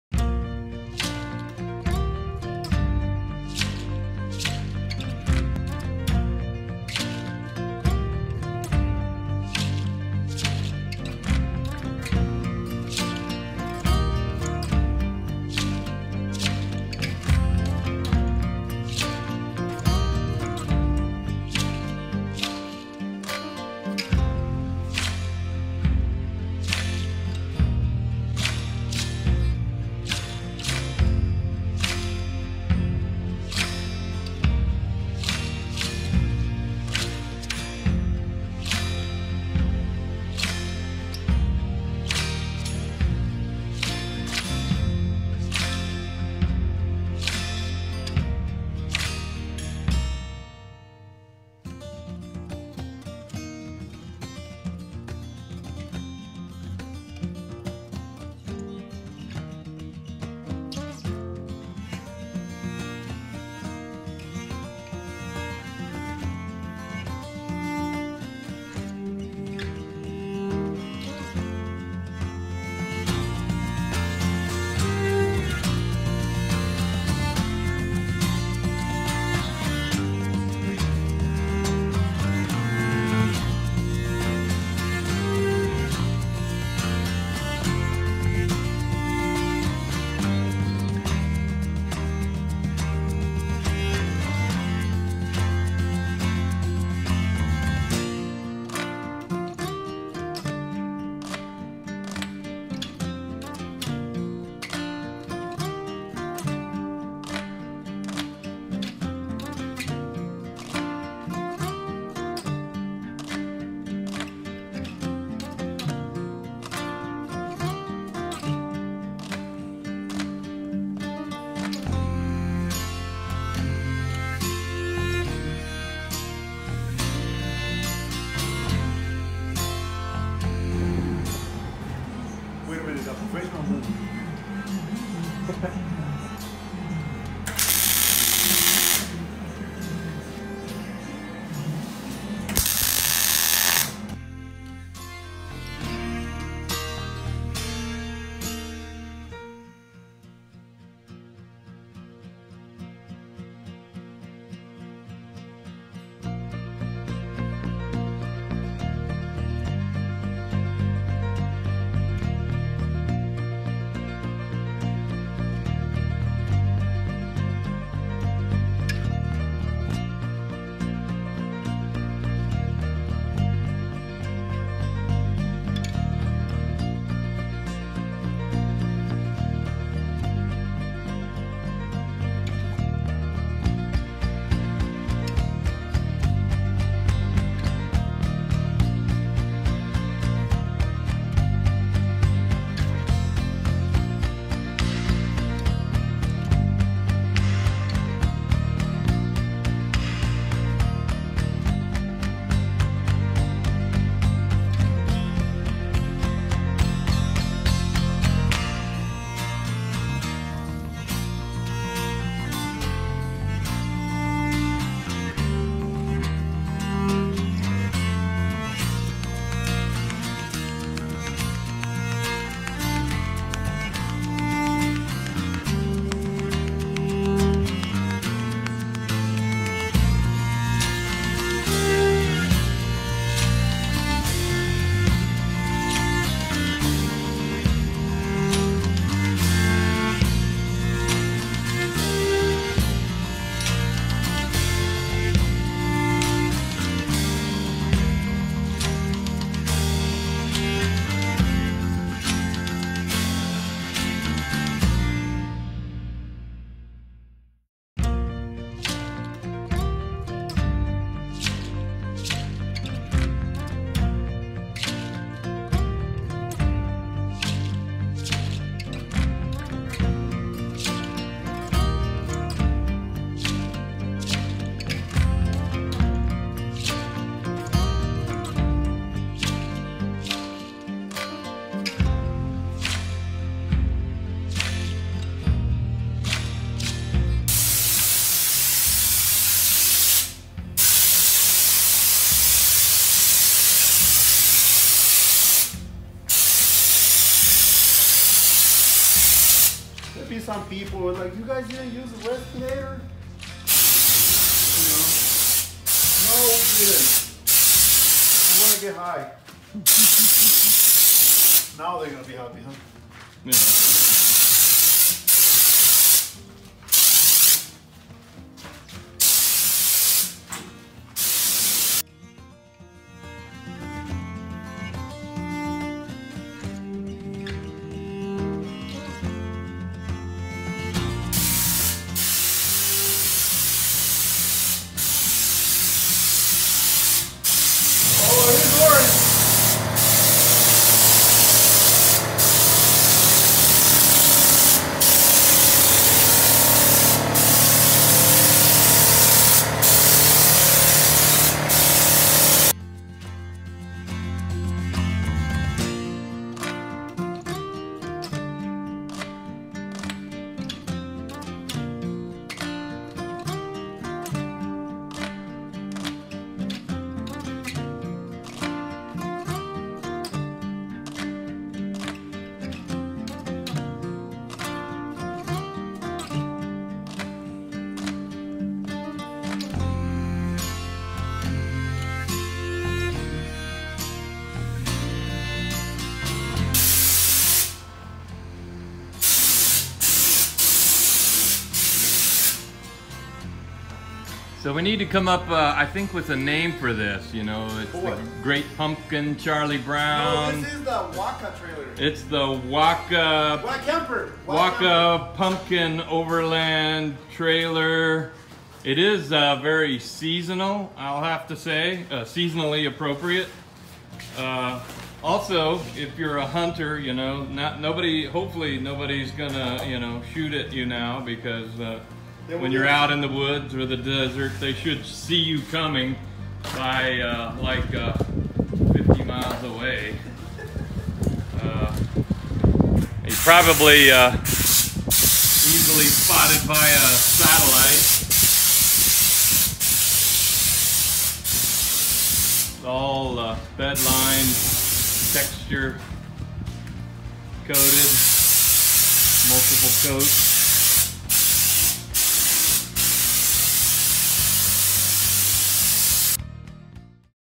Waka's Orangutan Overland Camping Trailer Slide Show
-Waka's Orangutan Overland Camping Trailer Slides
Still have Doors, hardware and some wiring before it is ready to go out.